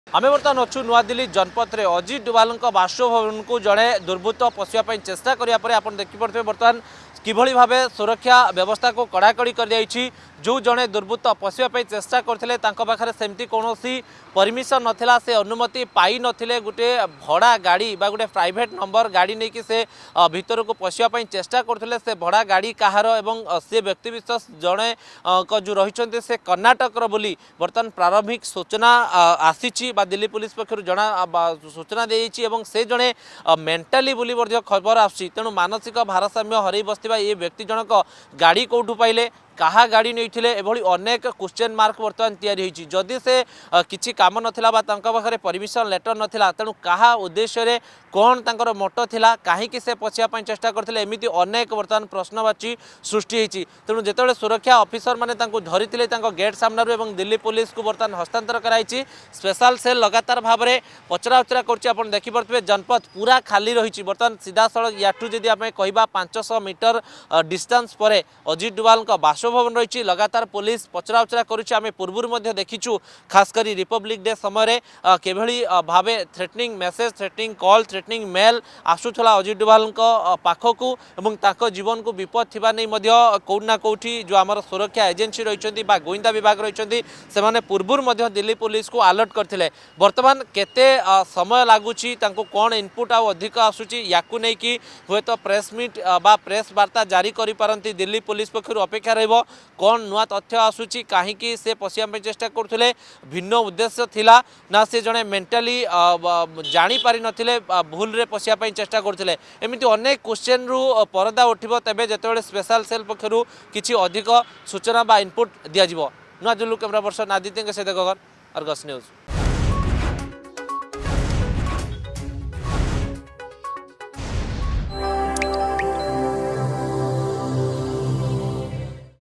Delhi: Man tries to enter National Security Advisor Ajit Doval's Residence
A man tries to enter National Security Advisor Ajit Doval's residence. Security forces have detained the man who tried to enter the residence of National Security Advisor (NSA) Ajit Doval in Delhi. The man appears to be mentally unsound, said Delhi Police.